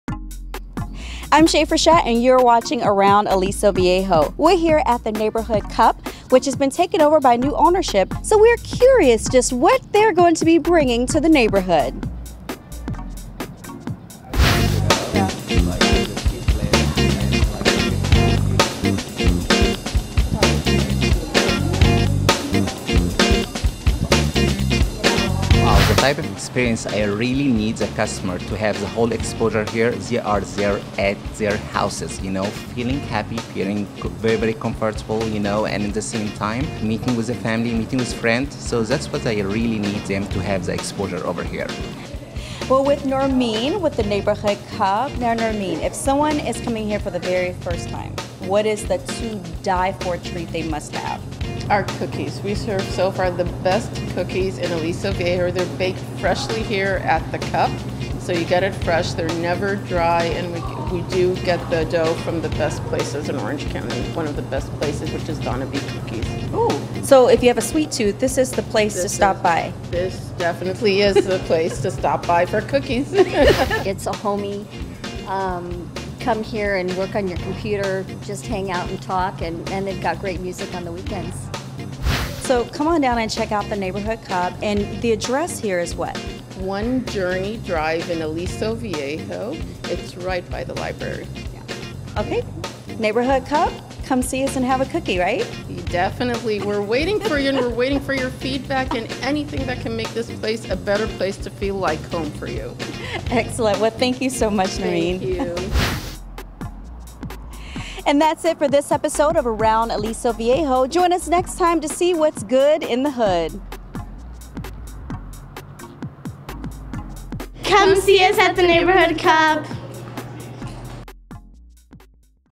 The Neighborhood Cup - Aliso Viejo, CA
- In December 2011, the Neighborhood Cup on 1 Journey Drive, Aliso Viejo changed ownership. Hear what the new owners want to provide to their fans.